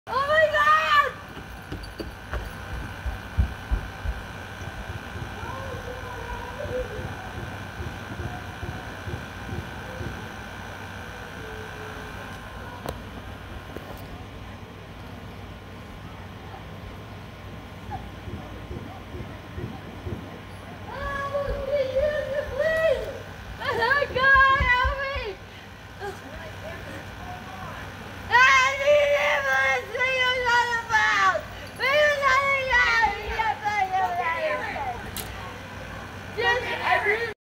Drunk asshole
My drunk asshole neighbor came home wasted. He pulled up almost against my friends car because he was parked in his spot (no assigned parking) walked on to the first landing fell backwards smashed his head